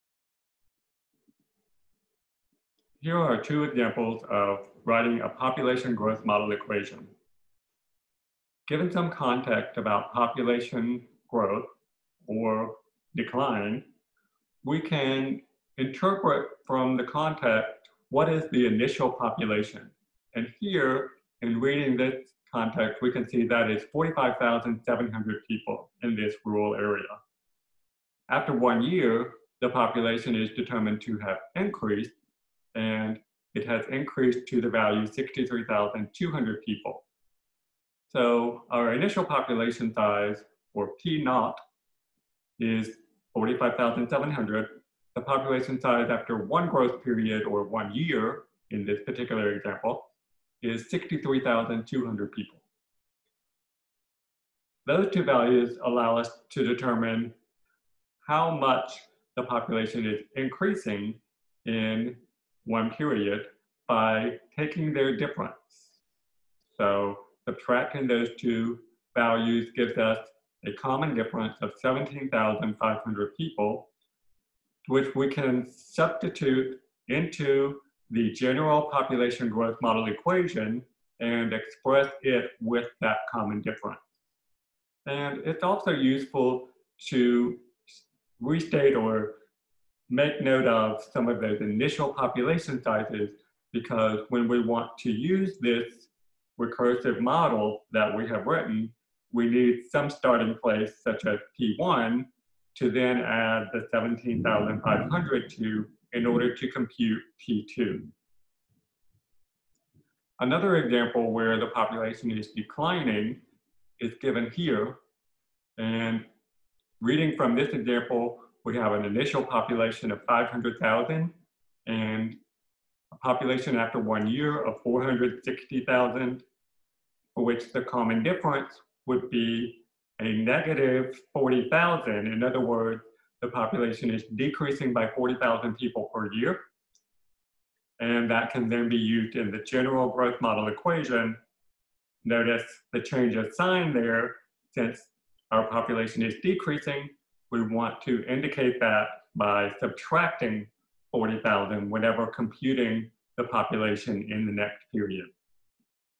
Population Growth Models
Write the equation for a population growth model given a context in which the initial population and the population after one period of growth are expressed.